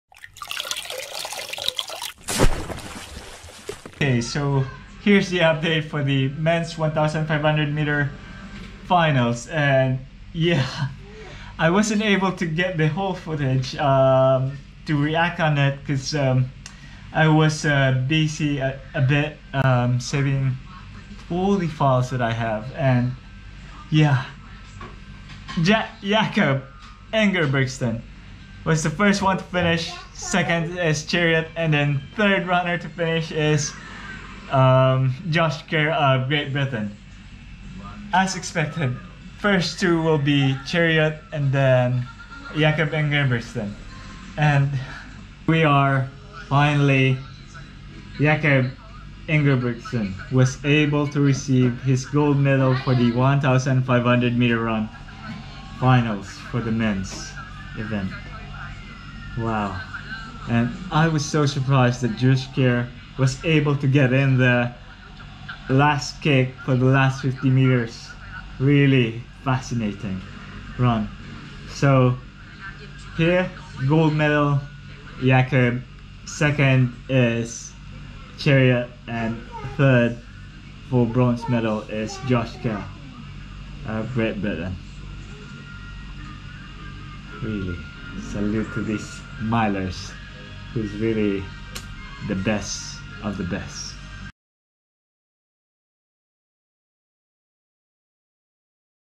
Men 1500m Finals Finish and Awarding | Tokyo Olympics 2020 Reaction
Men 1500m Finals Result:
1. NOR INGEBRIGTSEN Jakob - 3:28.32
2. KEN CHERUIYOT Timothy - 3:29.01
3. GBR KERR Josh - 3:29.05
4. KEN KIPSANG Abel - 3:29.56
5. ESP MECHAAL Adel - 3:30.77
6. USA HOCKER Cole - 3:31.40
7. AUS McSWEYN Stewart - 3:31.91
8. POL ROZMYS Michal - 3:32.67
9. GBR HEYWARD Jake - 3:34.43
10. GBR WIGHTMAN Jake - 3:35.09
11. AUS HOARE Oliver - 3:35.79
12. LUX GRETHEN Charles - 3:36.80
13. ESP FONTES Ignacio - 3:38.56